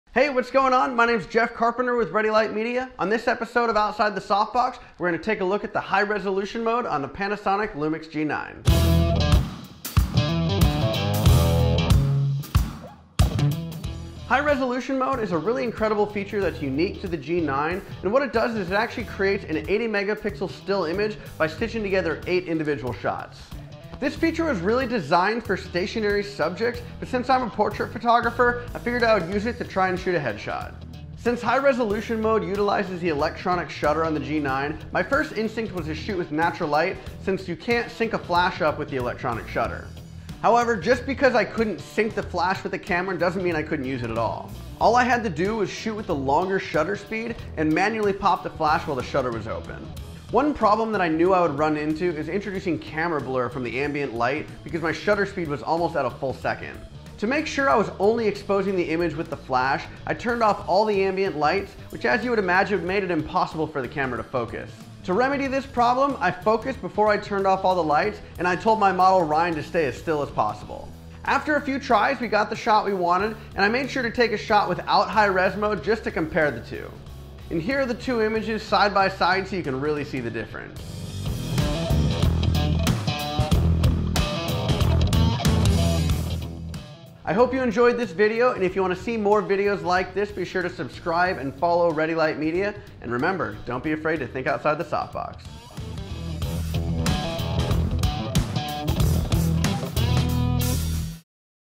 LUMIX G9 - Episode 3 Outside The Softbox with Jeff Carpenter - High Res Mode
What is "High Res" mode on the LUMIX G9? Its a whopping 80 MP still photo from a Micro Four Thirds sensor.

Watch Jeff Carpenter as he demonstrates how you can use the unique feature for portrait photography.